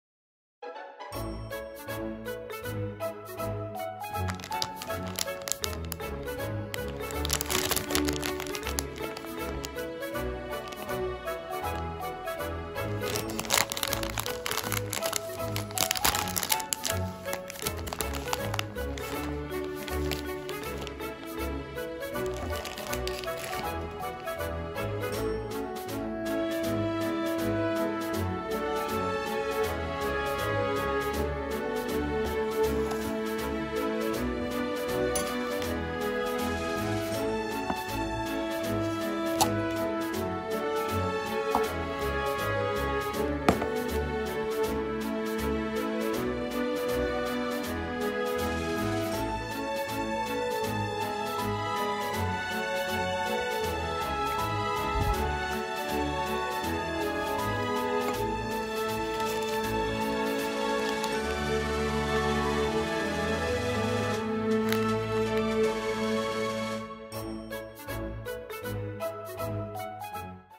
【スイーツ（JAPAN swees）】バナナカステラ（banana castella）「リマ」
バナナカステラ（banana castella）

140円（税込151.2円）
エフマートでの価格（2024年2月購入）

134kcal（1本あたり）